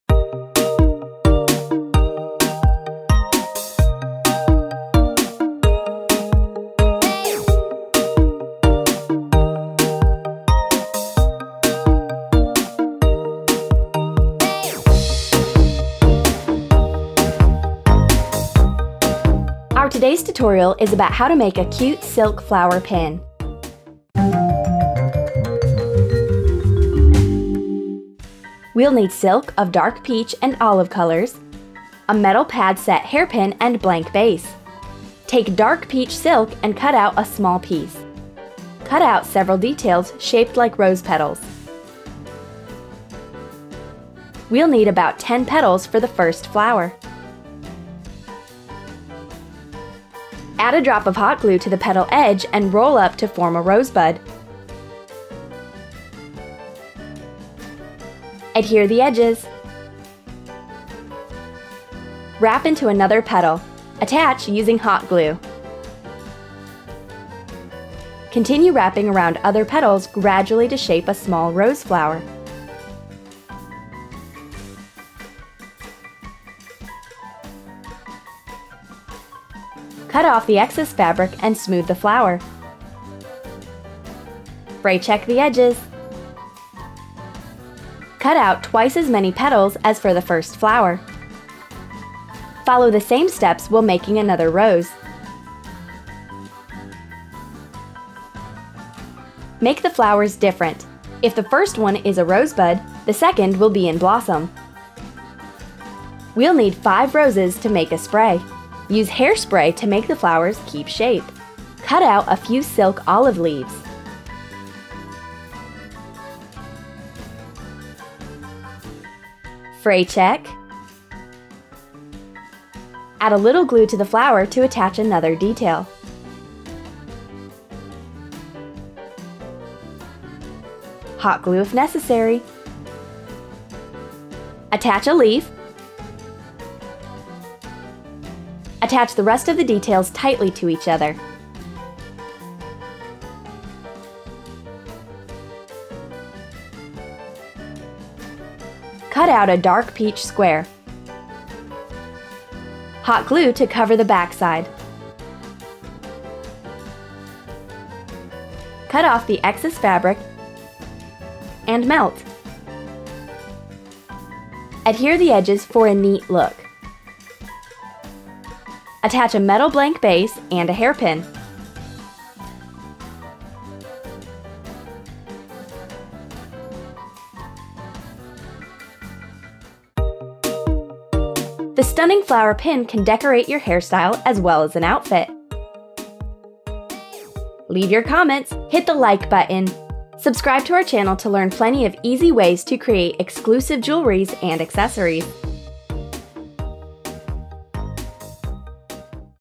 DIY Silk Flower Pin
How to make silk flower pin. In today's tutorial we'll craft a cute silk boutonniere. This stunning DIY fabric rose accessory can decorate your hairstyle as well as outfit!